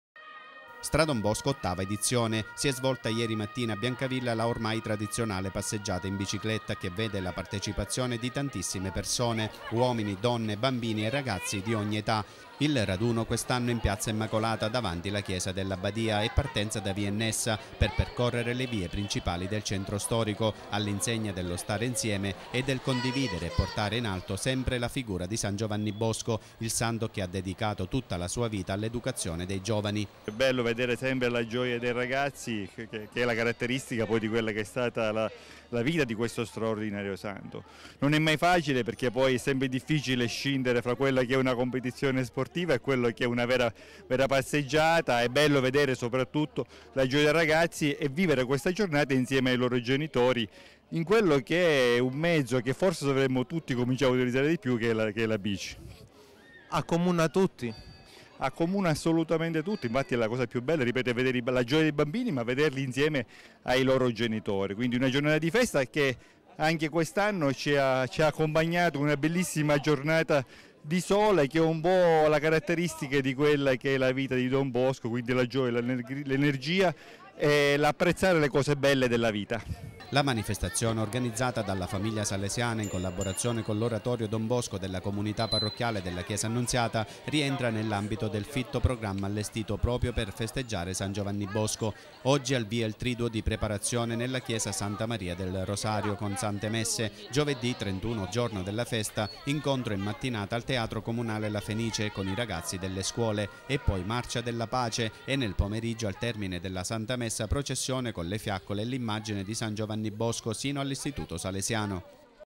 Biancavilla: StradonBosco 2019
Ottava edizione della passeggiata in bicicletta nelle vie del centro storico domenica 27 gennaio 2019